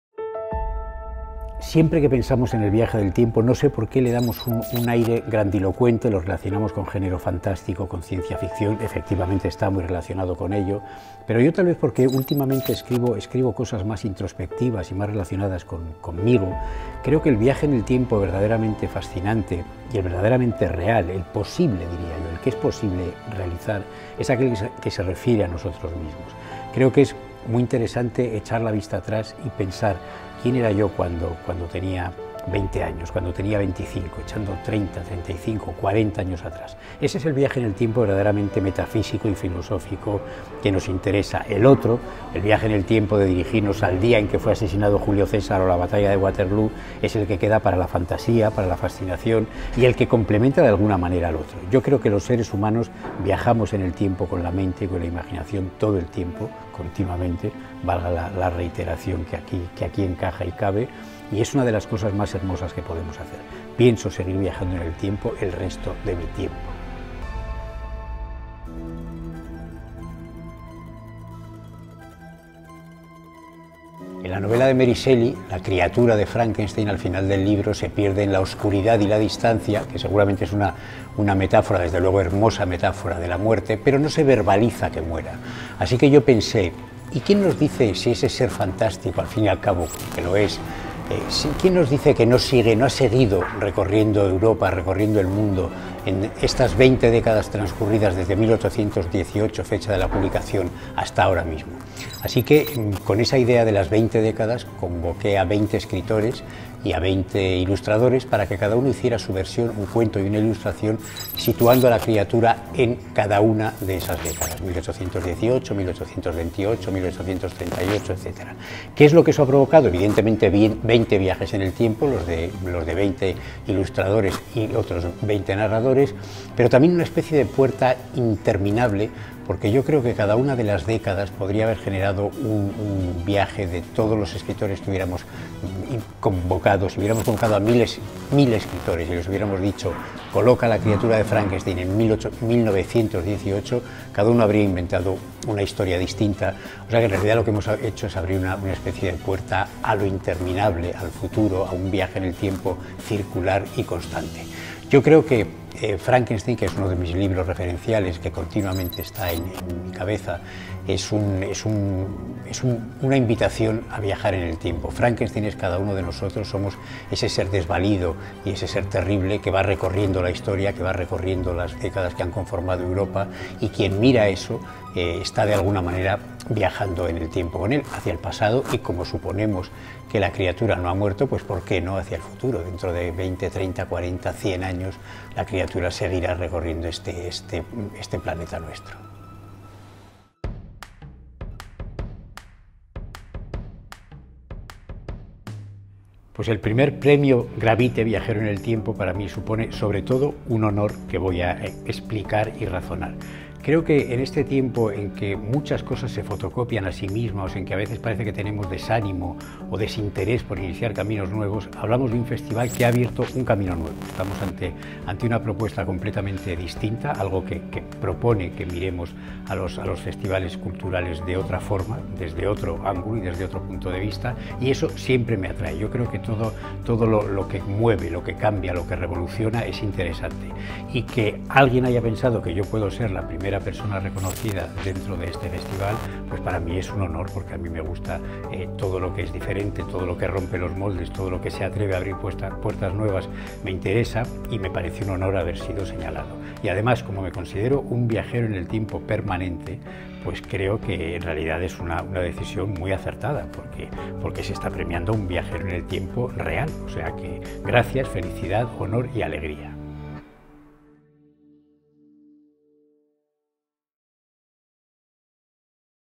Fernando Marías, I Premio Viajero del Tiempo del GRAVITE Festival
Este escritor y creador nato nos explica qué supone para él este galardón, por qué decidió emprender una travesía a través de los siglos con Frankenstein y con qué último periplo anda desafiando a Cronos.